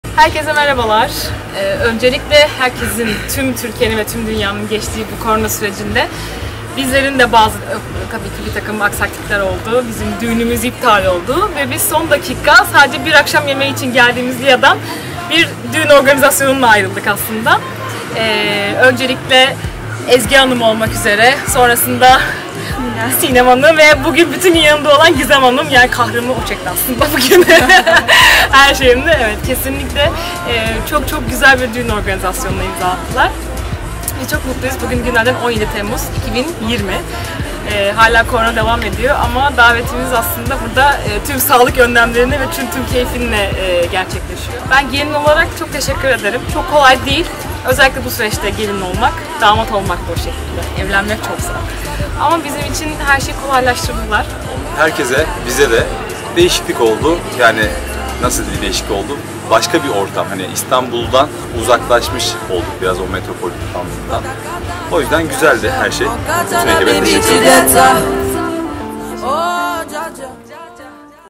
Çiftimizin Görüşleri-17.07.2020-Aybuke&Erdem
Pandemi yazında evlenen çiftimizin süreci nasıl anlattı :)